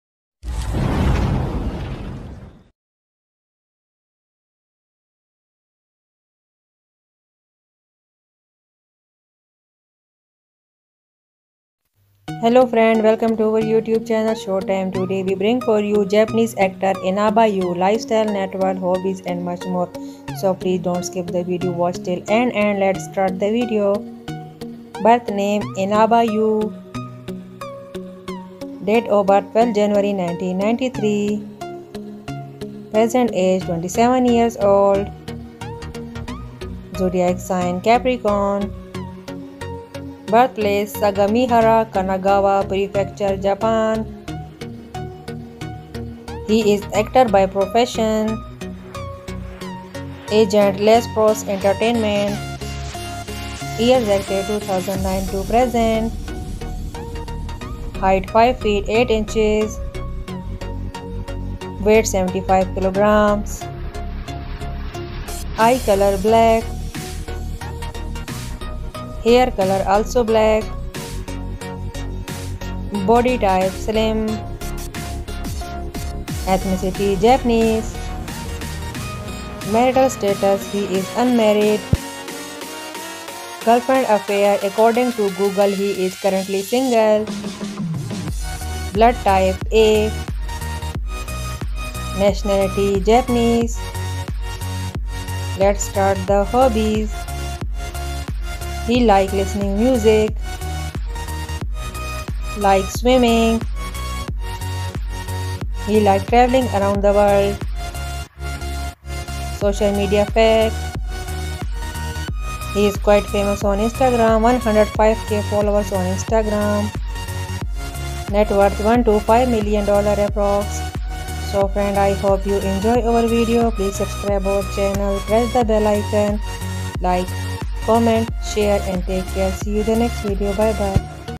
Inaba Yu Lifestyle (Shufu Maison) Biography, Net Worth, Real Age, Girlfriend, Height BY ShowTime
Yu Inaba Lifestyle 2021 (Shufu Maison) Biography, Net Worth, Real Age, Girlfriend, Height, Weight, Facts BY ShowTime

In November 2009, Inaba won the 22nd Junon Super Boy Contest Grand Prix that was submitted in the wake of a recommendation from the surrounding. He was a 16-year-old high school sophomore at the time. Inaba was awarded from among the largest number of 15,491 people. In 2010, he debuted in the TV drama Clone Baby. In 2011, Inaba first starred in the stage play Sanada Ten Braves: Boku-ra ga mamoritakatta mono.

TV Shows
Kokichi no Nyoubou 2 (NHK BS Premium, 2021)
38-sai Batsuichi Dokushin Onna ga Match-Making App wo Yattemita Kekka Nikki (TV Tokyo, 2020)
Byouin no Naoshikata (TV Tokyo, 2020)
Keiji to Kenji (TV Asahi, 2020, ep2)
Hatenkou Phoenix (TV Asahi, 2020)
Onzoshi Boys (AbemaTV, 2019)
Kinkyu Torishirabeshitsu 3 (TV Asahi, 2019, ep7)
Hana ni Kedamono~Second Season~ (FOD, 2019)
Yuube wa Otanoshimi Deshita ne (MBS, 2019)
Gakeppuchi Hotel! (NTV, 2018, ep10)
Ima kara Anata wo Kyohaku Shimasu (NTV, 2017, ep1)
Kyabasuka Gakuen (NTV, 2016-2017) cameo
Rental Kyuseishu (NTV, 2016)
Hikaru Gake (Fuji TV, 2016)
MARS (NTV, 2016)
Kamen Rider Drive (TV Asahi, 2014)
Clover (TV Tokyo, 2012)
Clone Baby (TBS, 2010)